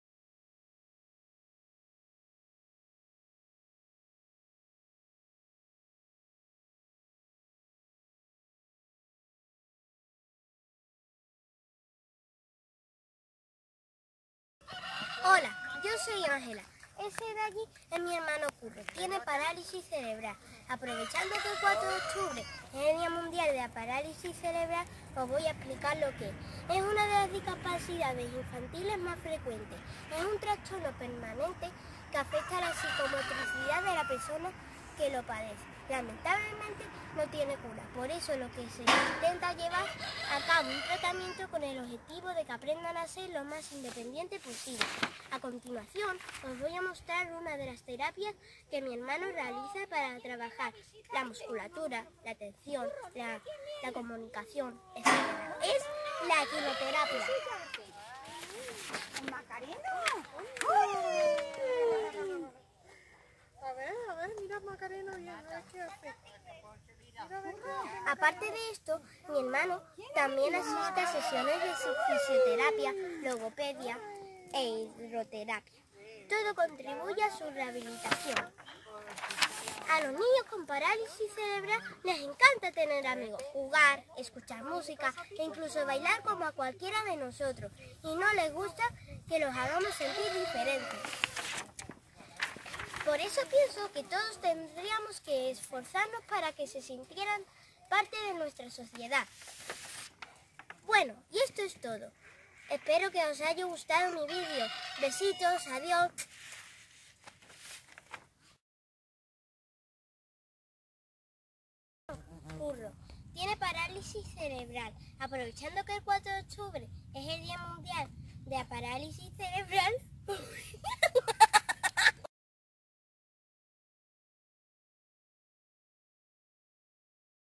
Día Mundial Parálisis cerebral - 4 Octubre
Una alumna del Colegio nos explica en que consiste la parálisis cerebral